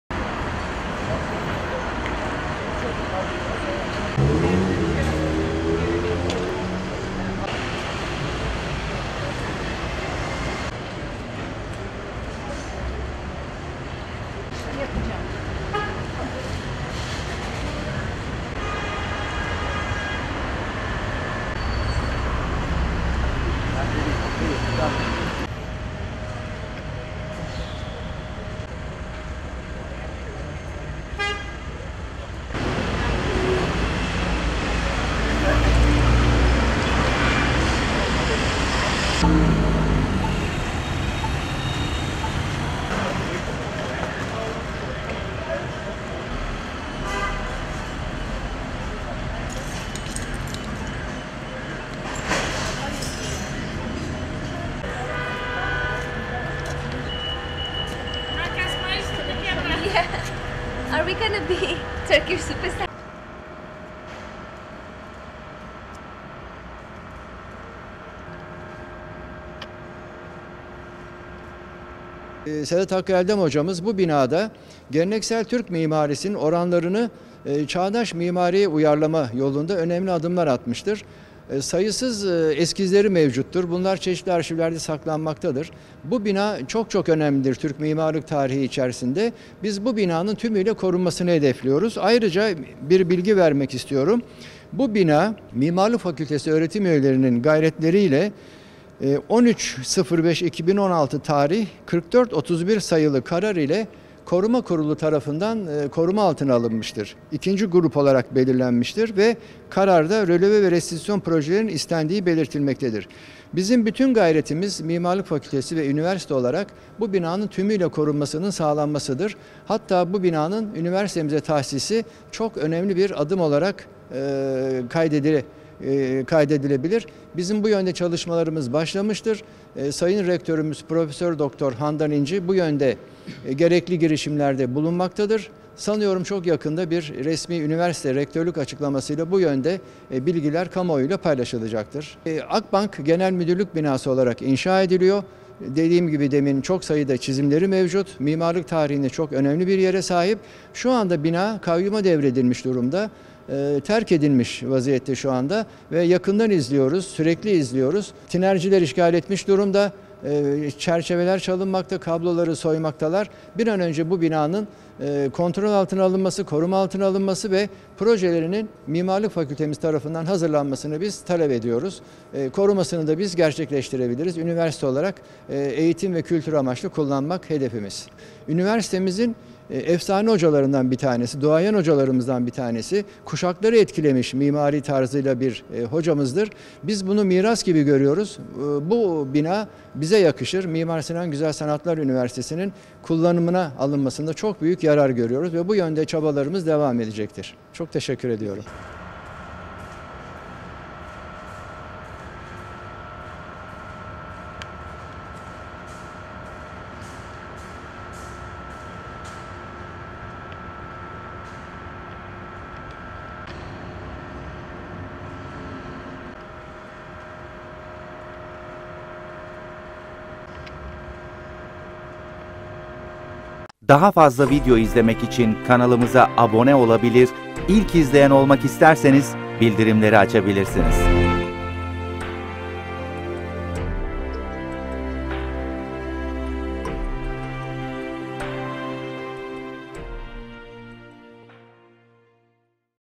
Beyoğlu’nda içler acıtan görüntü!
Beyoğlu’nda bulunan 53 yıllık tarihi bina hırsızların ve tinercilerin meskeni haline geldi. Mimar Sedad Hakkı Eldem’in çizdiği binanın bu hale gelmesinden duydukları rahatsızlığı dile getiren Mimar Sinan Güzel Sanatlar Üniversitesi öğretim üyesi Prof. Dr. Oğuz Ceylan, “Tinerciler işgal etmiş durumda. Çerçeveler çalınmakta, kabloları soymaktalar. Biz bu binanın tümüyle korunmasını hedefliyoruz” dedi.

Beyoğlu Meclis-i Mebusan Caddesi üzerinde yer alan ve 1968 yılında inşa edilen eski Akbank Genel Merkezi binası bir süredir terk edilmiş halde bulunuyor. Yapıldığı dönemde çağın ötesinde bir mimari çizgiye sahip olduğu kabul edilen bina, şimdilerde hırsızların ve tinercilerin mesken tuttuğu bir yer haline geldi. Binanın yanından geçenler İstanbul’un orta yerinde gördükleri bu kötü görüntüye anlam veremezken, kimi vatandaşlar ise binadan dökülen parçaların tehlike oluşturduğunu ve acilen tedbir alınması gerektiğini söylüyor. Mimarisiyle göz kamaştıran tarihi binanın çiziminin Mimar Sinan Güzel Sanatlar Üniversitesinin eski hocalarından, 1988 yılında hayatını kaybeden Sedad Hakkı Eldem tarafından yapıldığı biliniyor. Sedad Hakkı Eldem gibi değerli bir mimar ve eğitimcinin eserinin talan edilmesi ve kaderine terk edilmesine anlam veremediklerini söyleyen Mimar Sinan Güzel Sanatlar Üniversitesi Öğretim Üyesi Oğuz Ceylan, şu an kayyumun elinde olan yapının üniversiteye tahsis edilerek eğitim ve kültür amacıyla kullanılmasını istediklerini dile getirdi.